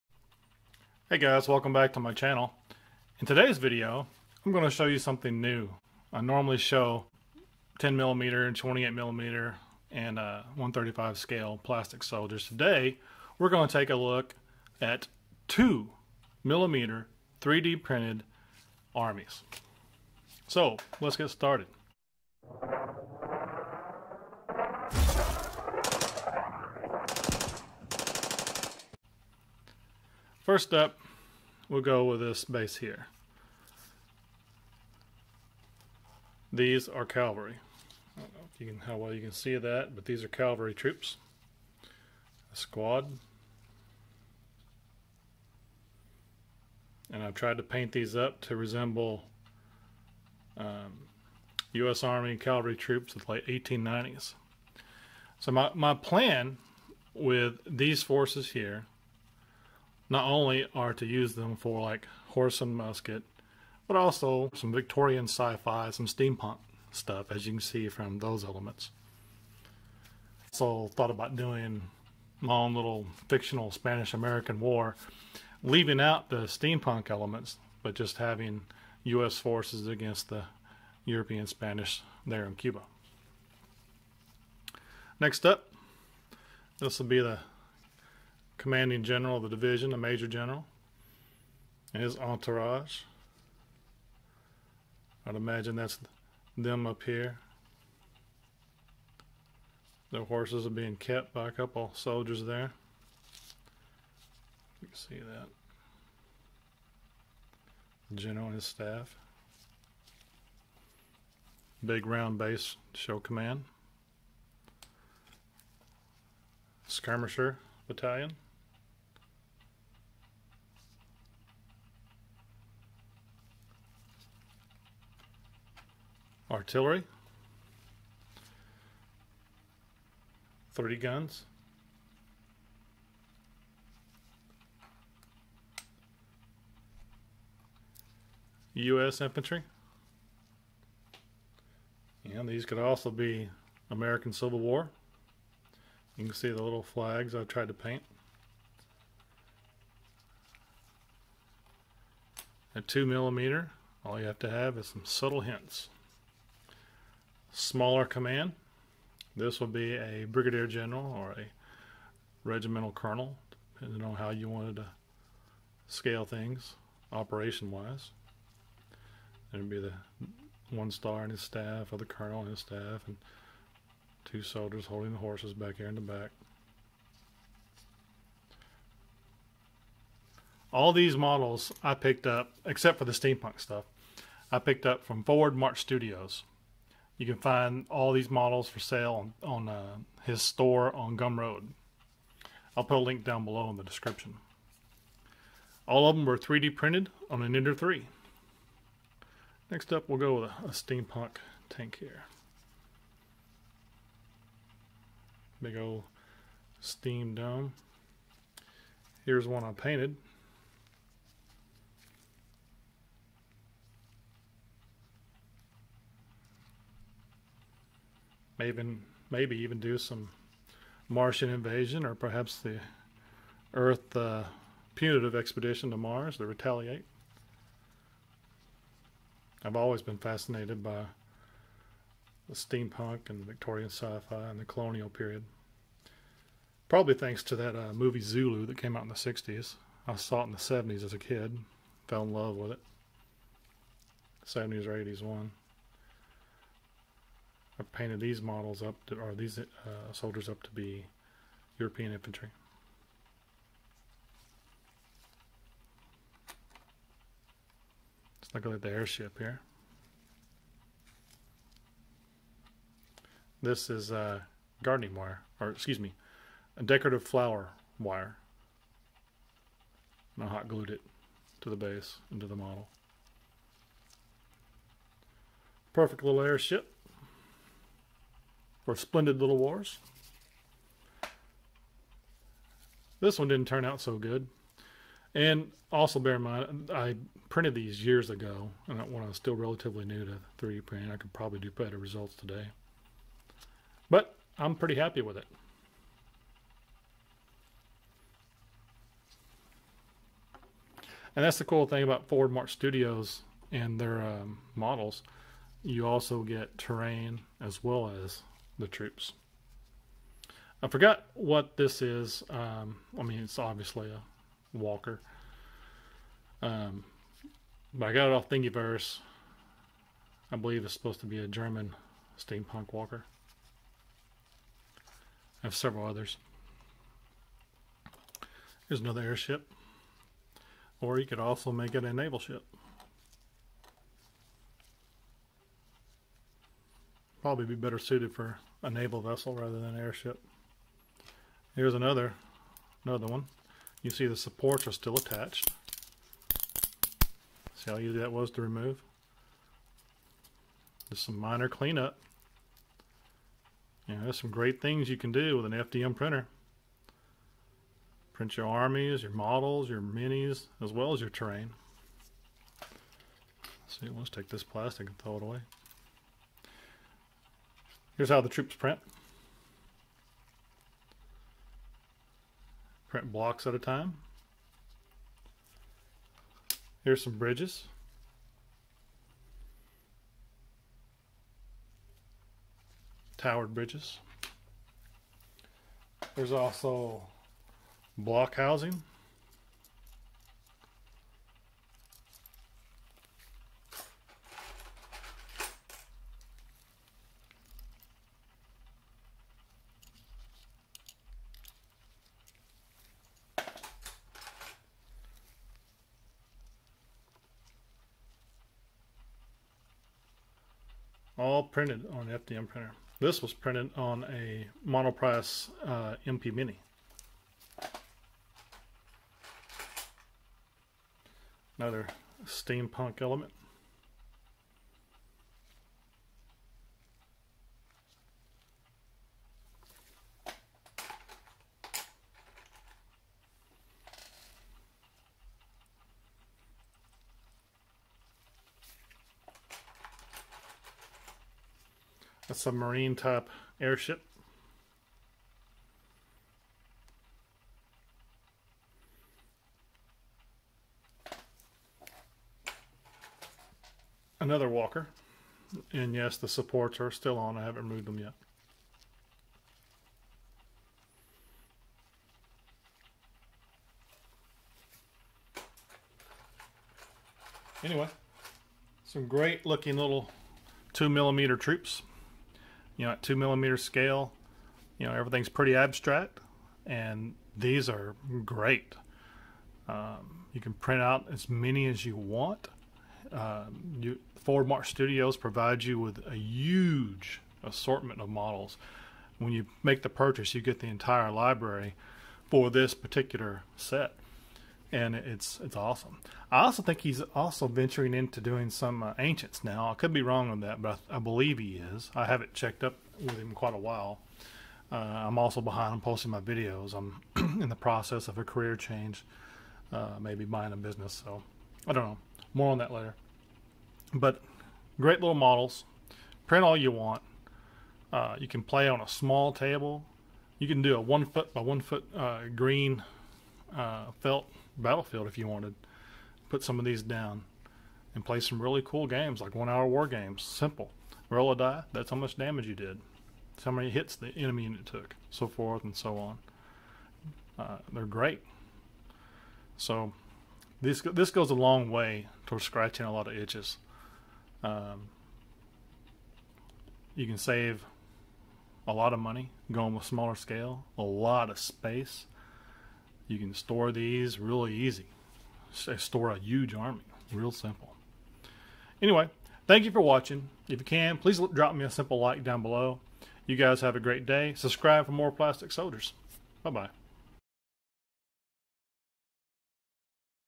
3D PRINTED 2mm WARGAME MINIATURES | FORWARD MARCH STUDIOS | MOOSEWORKS PLASTIC SOLDIERS
In today's video I'm going to show you some 3D printed 2mm wargame miniatures I made with my Ender3. Most of these models were designed by Mr. Zach Duckworth of Forward March Studios (link below). The VSF elements were downloads from Thingiverse. Zach has made some great models and I look forward to printing more of these and picking up his new lines when they grow.

Forward March Studios

MOOSEWORKS PLASTIC SOLDIERS